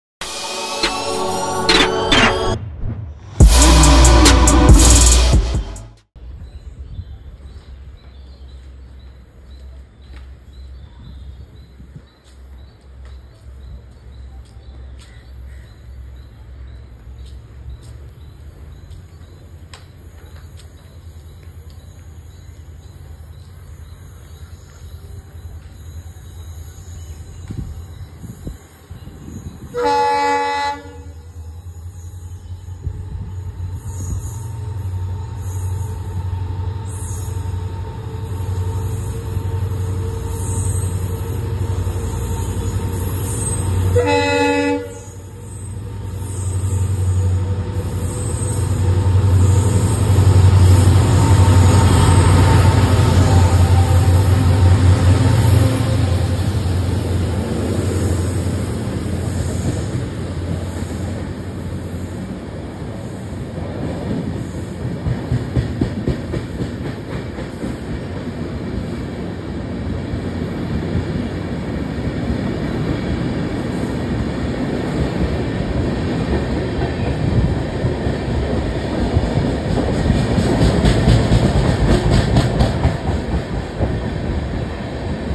LKO WDG4D Twins powering 09207 Bandra Terminus Bhavnagar Special Fare Train | Indian Railways | EMD
Indian railways train compilation
Indian railways locomotives
Indian railways diesel locomotives